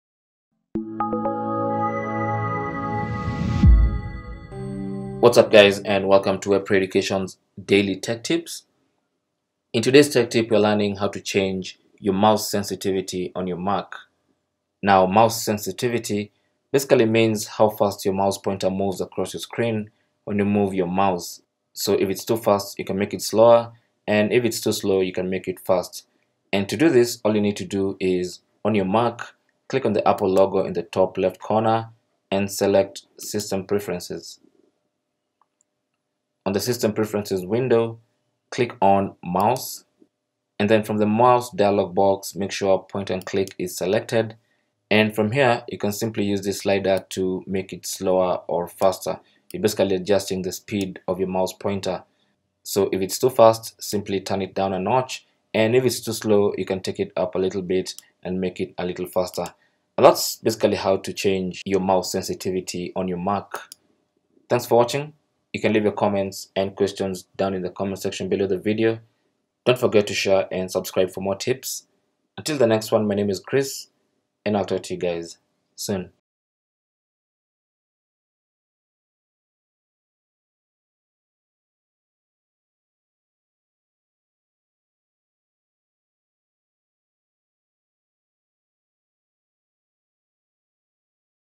How To Change Mouse Sensitivity On Mac
Learn how to change your Mouse DPI in your Mac computer settings. DPI means 'dots per inch  which essentially refers to the mouse sensitivity.

How To Change DPI On Mouse:

Go to system preferences
Select Mouse settings
Go ahead and adjust the mouse sensitivity using the slider.
Exit the preferences and try out your adjusted mouse sensitivity.